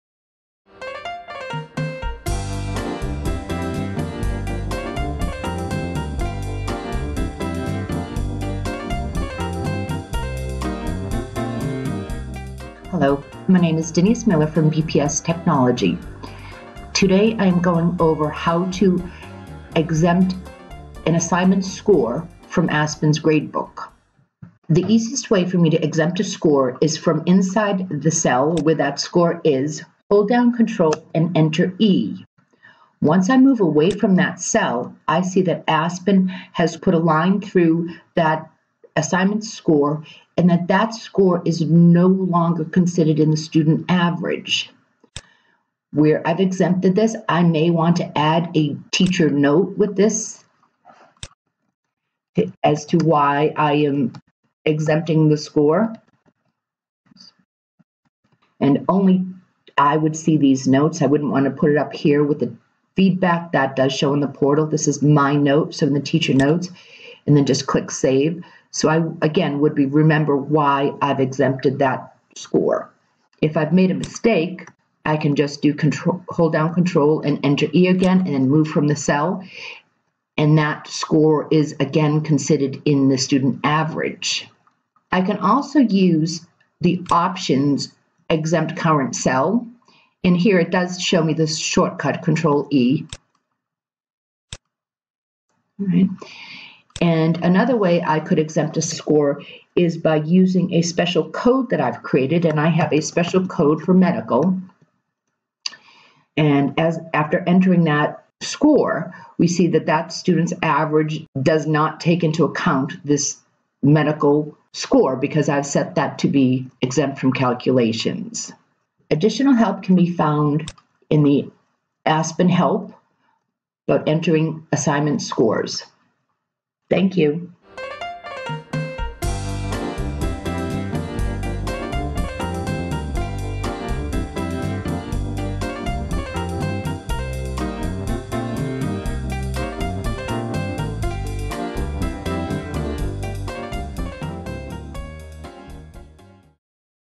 Gradebook: How to Exempt a Score in Aspen
Quick Aspen Video Tip: How to Exempt a Score in Gradebook

Sign up for the Monthly Digital Learning Newsletter email and click JOIN GROUP!